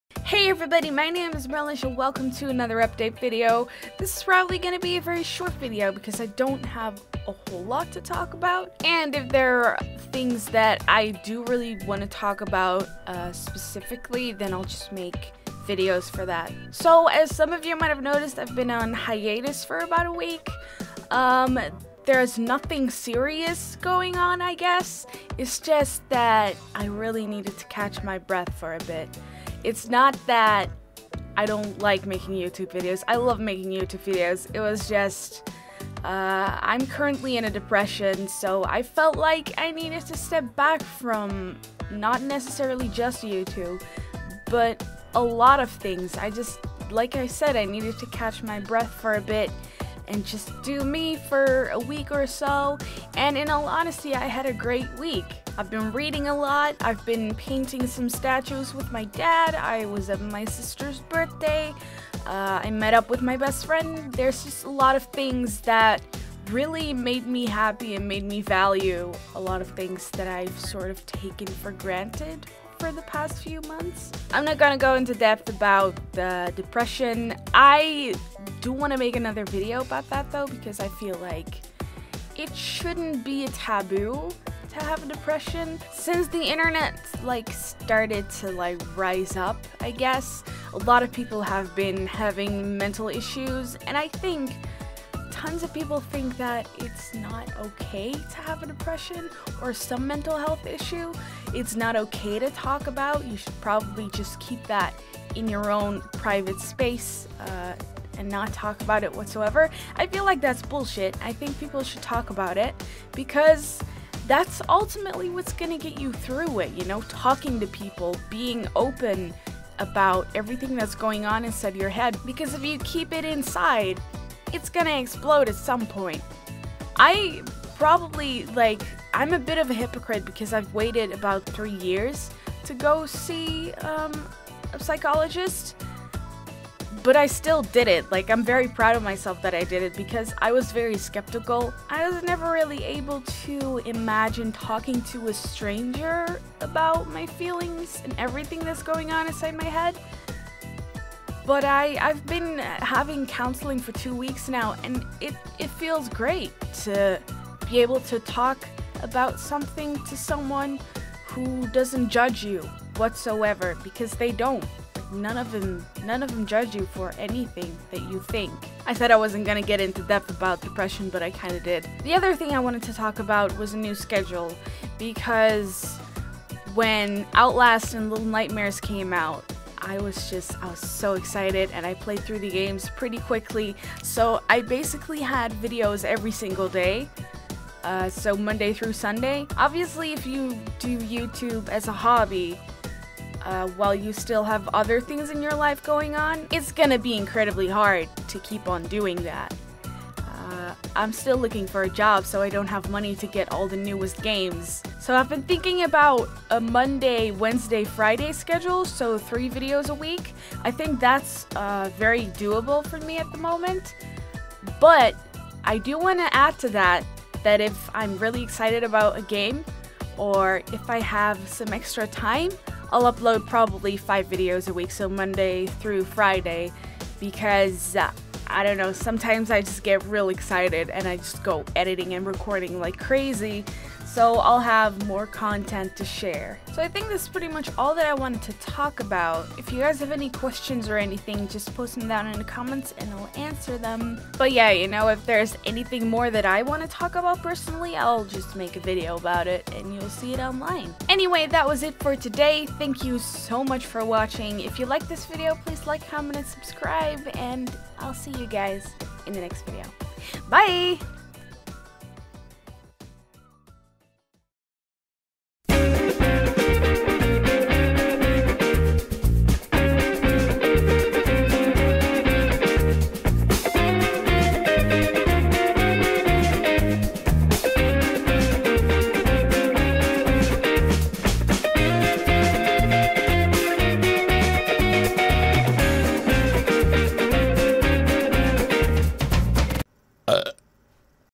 Update - Back at it again with a new schedule!
Hey guys! Welcome to another update!
I'll be explaining what I've been up to and announce a new upload schedule!